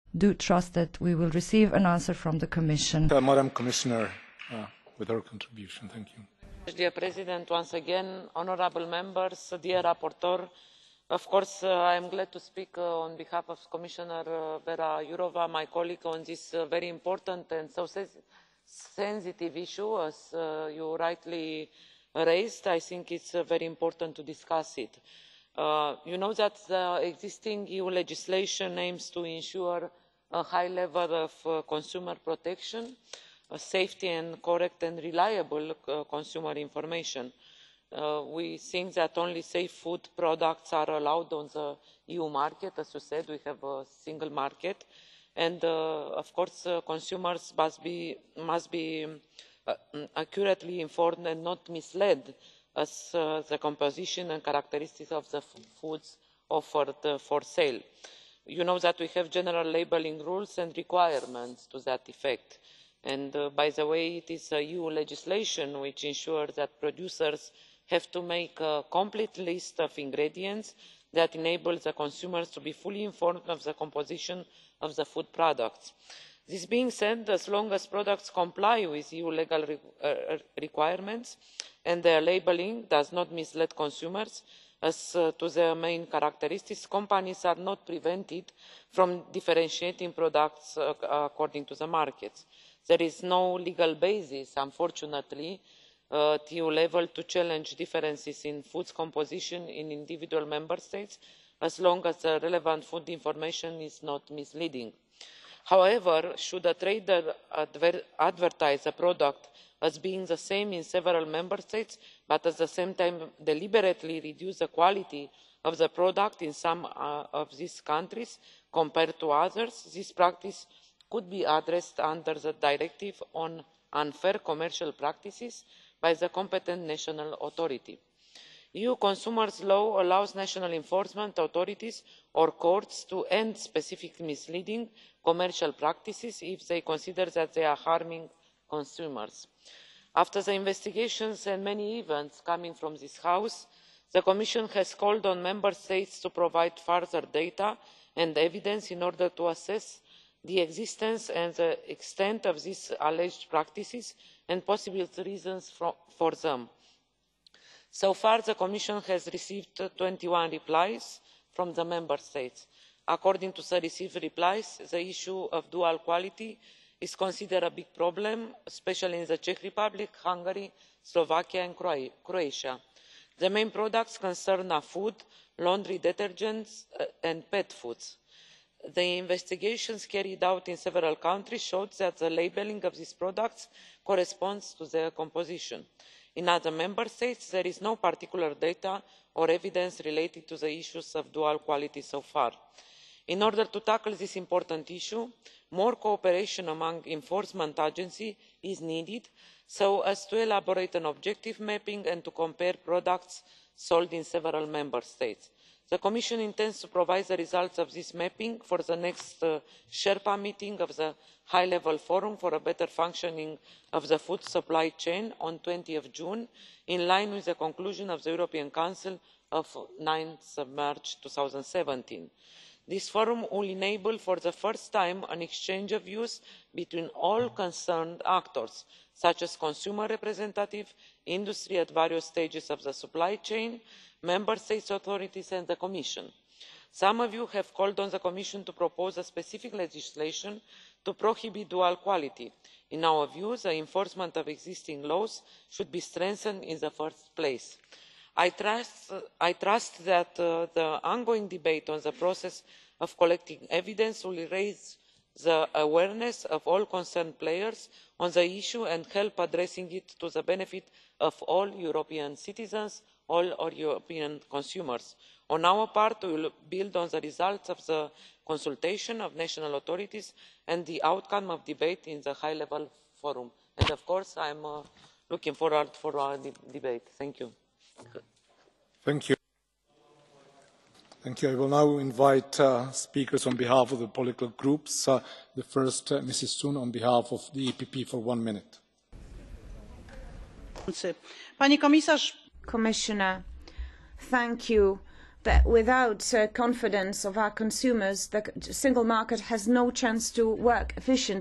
Cssr Corina Crețu in the EP plenary - Major interpellations, Consumer Protection
European Parliament Plenary Session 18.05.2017
Differences in declarations, composition and taste of products in Central/Eastern and western markets of the EU